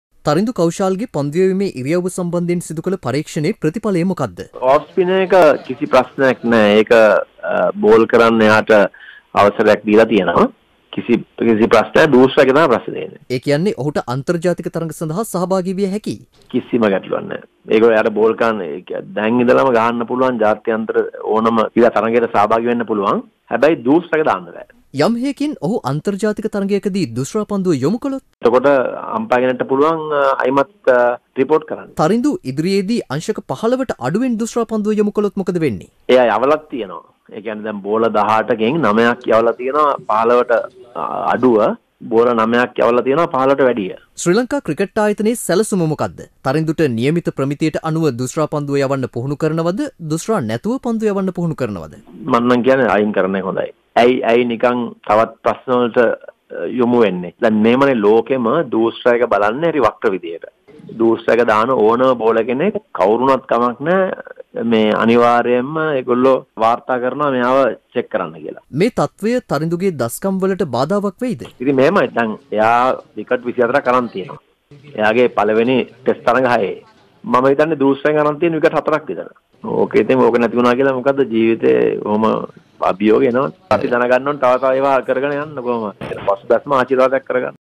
Tharindu Kaushal's Doosra Ban What will Next? (KeliBime Keli )
තරිදු කෞෂාල්ට මොකද වෙන්නෙ?

පන්දු යැවීමේ තහනමක් නෑ - දුස්රා තහනම් කියලයි අයි සී සී එක කියන්නේ තරිදු කෞෂාල්ට ඊළගට මොකද වෙන්නෙ?
ලක්හඩ ඇහුවා ශ්‍රී ලංකා ක්‍රිකට් ආයතනයෙන්.

තරිදු පරීක්ෂණයේදී යැවූ දුස්රා පන්දු 18න් 9ක්ම අංශක 15ට වඩා අඩුවෙන් පන්දුව යවා තිබෙනවා.ඉතිරි පන්දු 9 පමණයි අංශක 15ට වැඩියෙන් සටහන් වී ඇත්තේ.